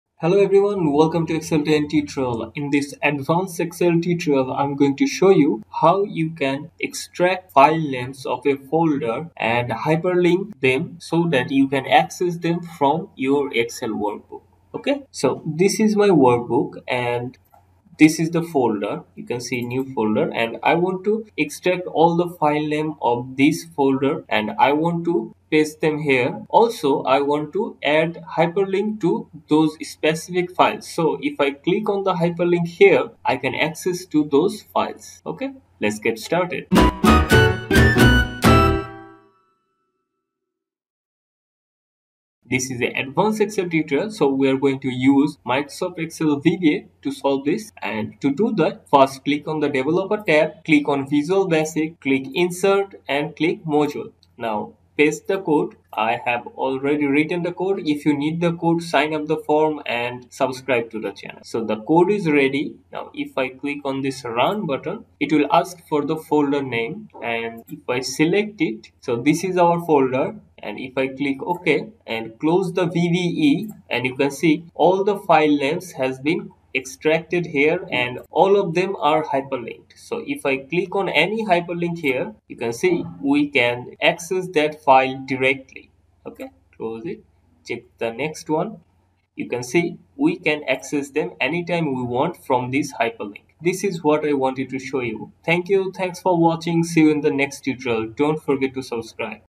How to List all Filenames of a Folder and Create Hyperlinks to Them in Excel
In this advanced excel tutorial we are about to create hyperlinked list of files in folders using vba in Microsoft excel. This tutorial will help you to create hyperlinked file directory inside your worksheet. This could be of tremendous help if you understand the scope of this tutorial and follow the instructions correctly. I’m using Microsoft Excel 2019 for the demonstration and we’ll be using vba to solve this. Let’s get started.

To use macro to create list of hyperlinks to all files in a folder follow the instructions as stated.

Step 5:  Click On the "Run" Button
Step 6: Select the folder from your computer
Step 7: Click “OK” & Close the VBE

Done. You just created hyperlinks to all files in a folder. So using excel vba and hyperlink to files in a folder isn’t that hard. right?